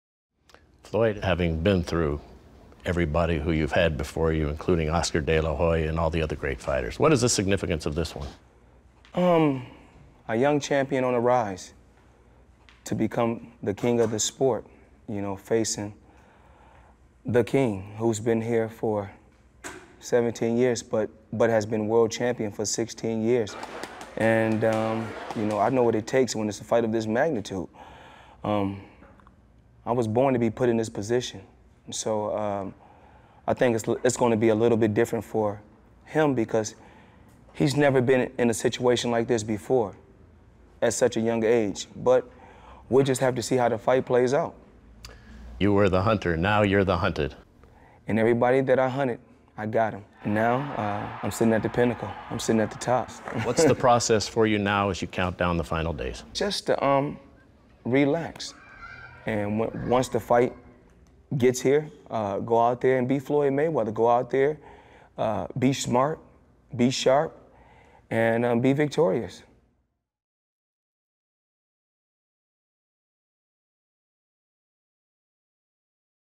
Floyd Mayweather: An Interview with Jim Gray - SHOWTIME Boxing
An interview with the undefeated pound-for-pound king, Floyd "Money" Mayweather.